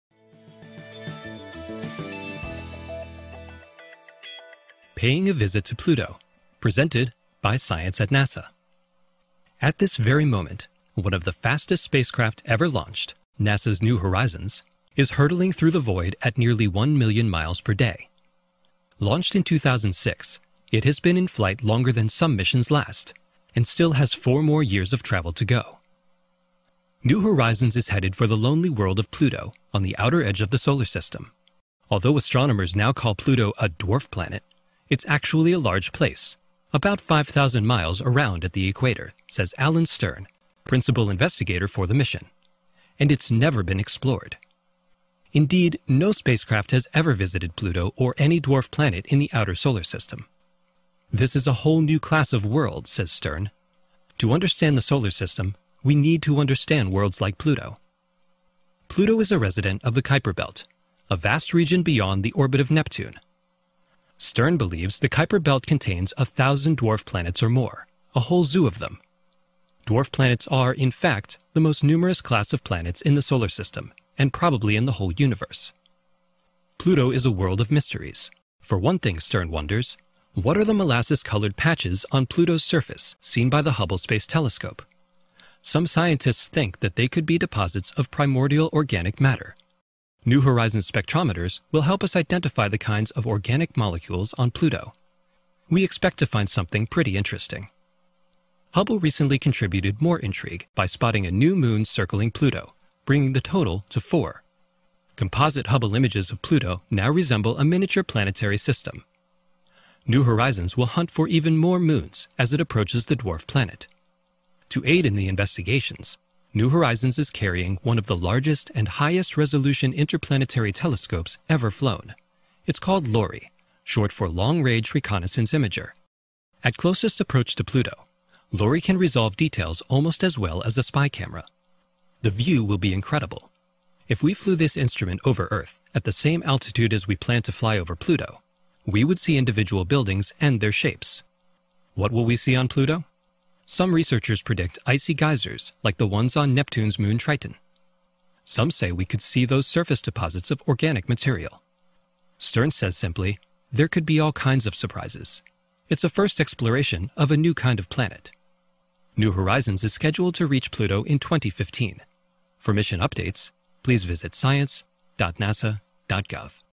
Il viaggio verso Plutone della sonda New Horizons
Lanciata il 19 gennaio 2006, la sonda New Horizons arriverà ai margini del sistema solare per studiare Plutone e il suo satellite Caronte nel Luglio 2015 per poi proseguire il suo viaggio verso la fascia di Kuiper.

(solitamente la qualità sul nostro sito è maggiore di quella offerta da YouTube!).